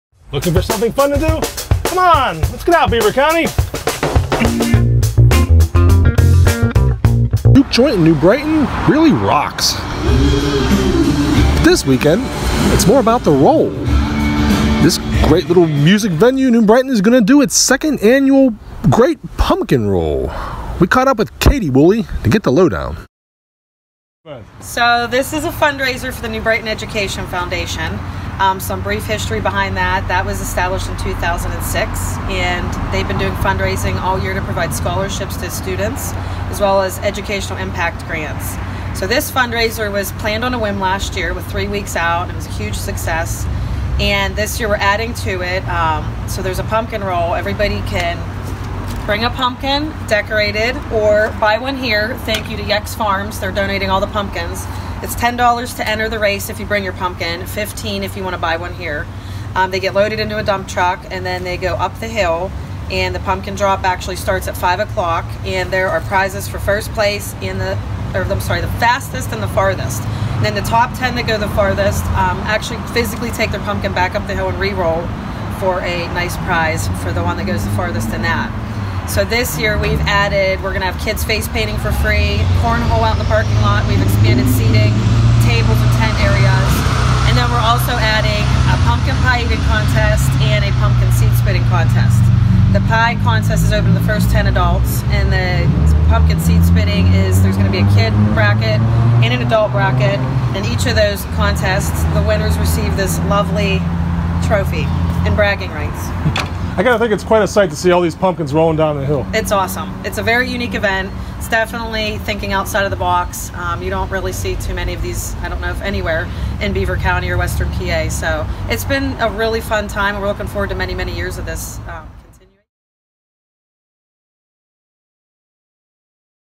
Get Out This Weekend: Great Pumpkin Roll
The second annual Great Pumpkin Roll will take place at 5 p.m. Saturday outside Wolley Bullys in New Brighton.